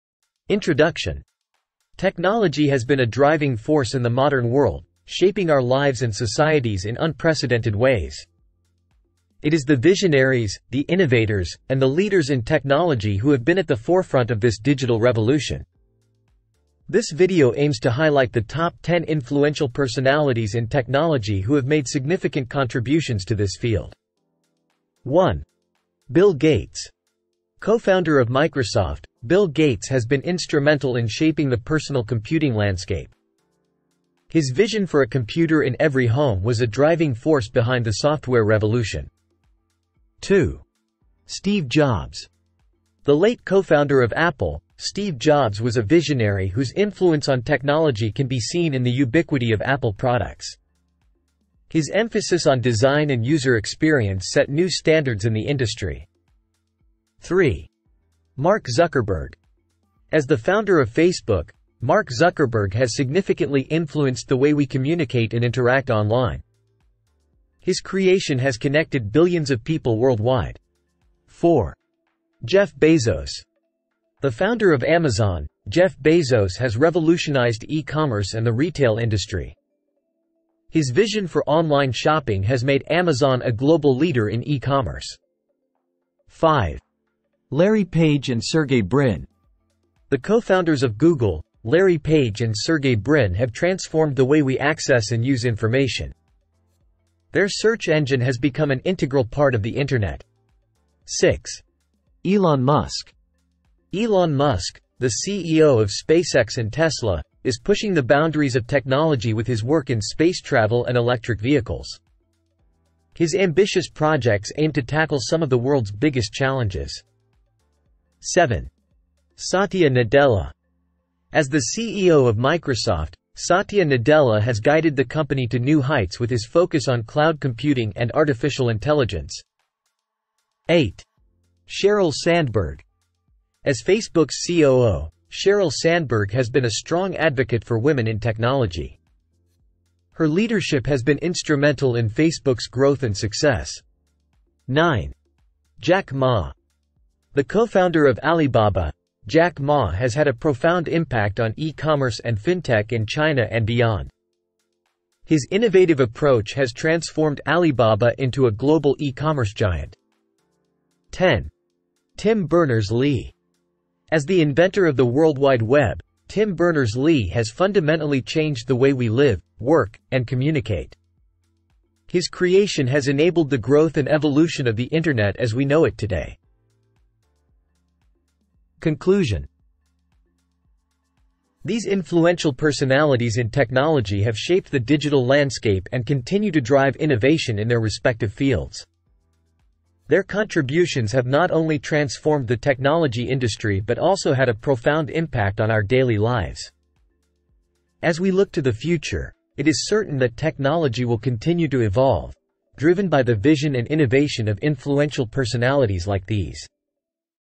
Top 10 Influential Personalities in Technology
Discover the visionaries shaping the tech landscape with this countdown of influential personalities who have left a significant mark on the world of technology.